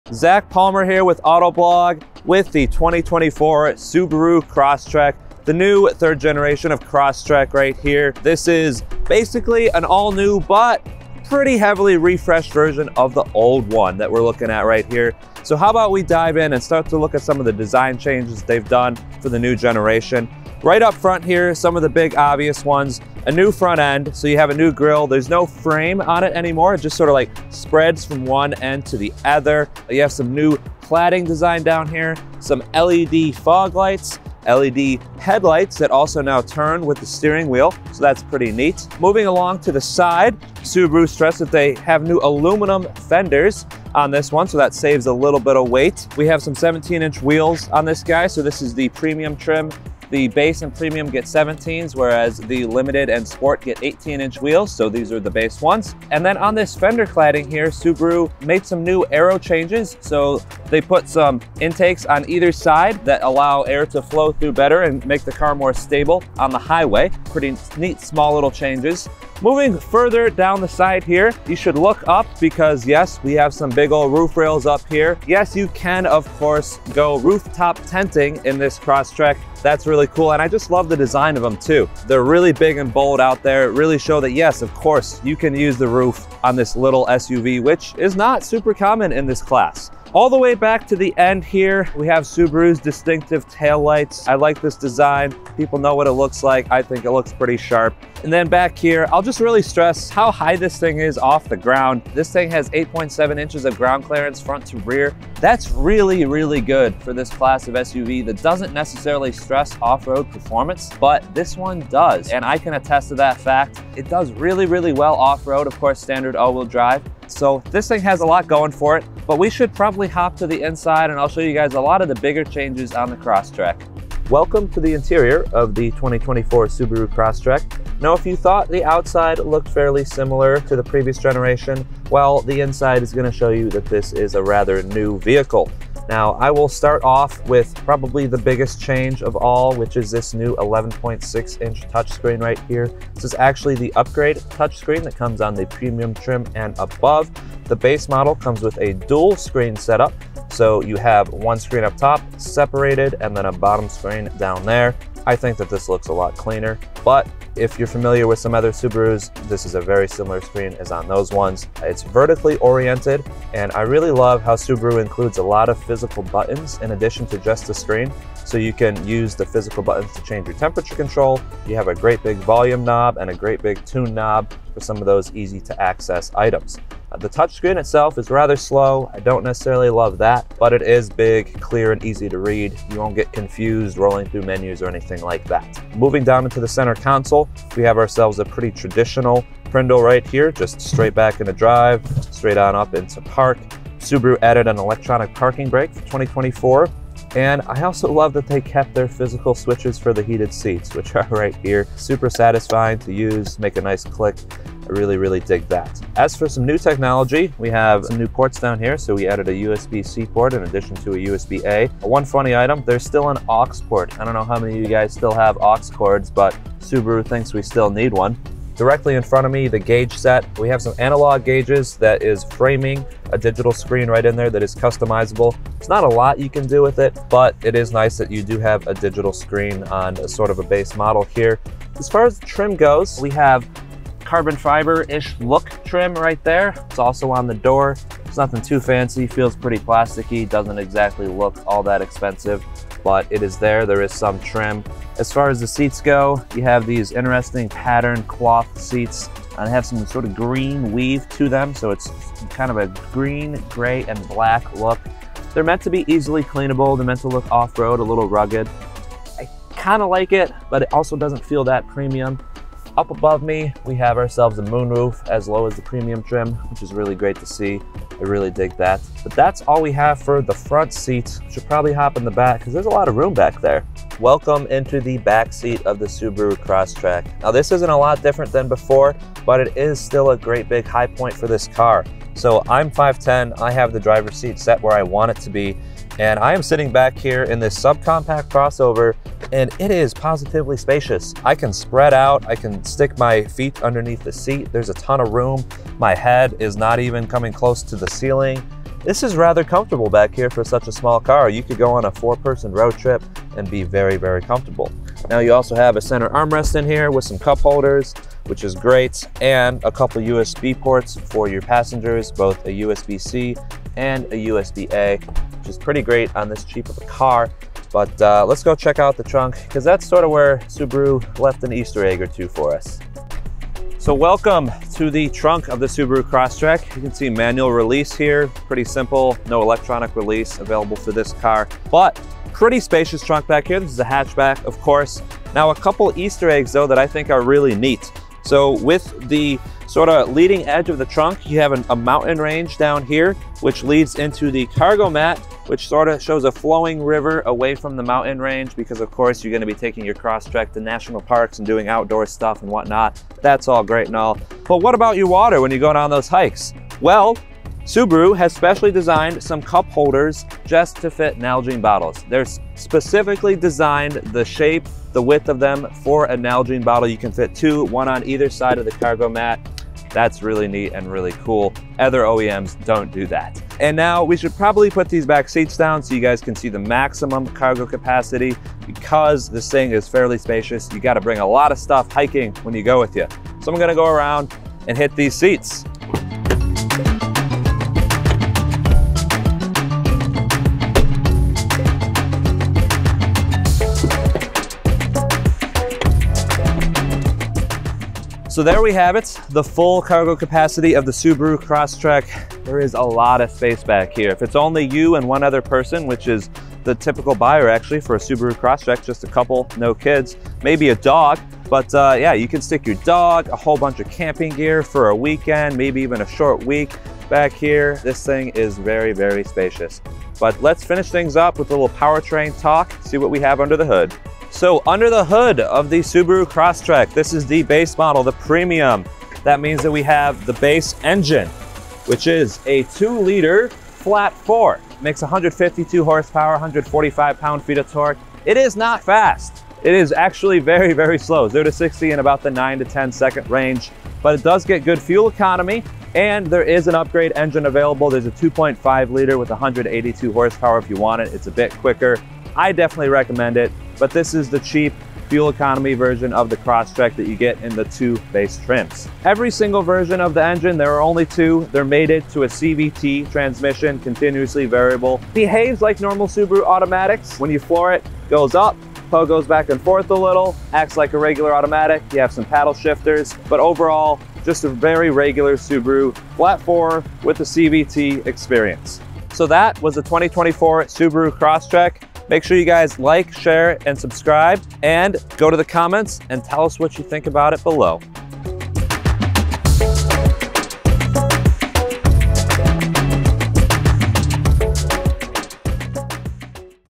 2024 Subaru Crosstrek Premium interior and exterior walkaround review | Autoblog Garage
Road Test Editor Zac Palmer gives us a detailed walkaround of the 2024 Subaru Crosstrek Premium.

Autoblog contributor Mark Takahashi wrote of the 2024 Subaru Crosstrek Premium:

Subcompact SUVs are gaining in popularity and for good reason. They deliver the elevated ride height and cargo capacity that so many SUV drivers desire, along with the kind of maneuverability, fuel efficiency and lower price expected of smaller sedans or hatchbacks. These pint-sized utes are a great alternative to larger SUVs, but if you're also interested in straying off the pavement, one of the best choices has been the Subaru Crosstrek. Now there’s a new one.

We had the opportunity to drive the redesigned 2024 Subaru Crosstrek through California's low desert to see for ourselves how this new, third-generation model performs as both a daily commuter and a rugged adventure vehicle. The 2024 Crosstrek shares the same platform as the second generation, but this time around, the chassis benefits from a 10% increase in structural rigidity, which improves safety, handling, and comfort.

From the outside, the 2024 Crosstrek doesn't break new ground in terms of styling (much like the related 2024 Subaru Impreza). It retains the lifted stance, trapezoidal grille, and tapering roofline from before but adds a bit more rugged personality with chunkier black plastic elements that flank the larger and frameless grille. Exterior dimensions are almost identical to the last Crosstrek too, with only a fraction of an inch difference here or there. On top is a substantial roof rack that can carry as much as 176 pounds when in motion or up to 700 lbs if you're parked and want to set up a roof-top tent.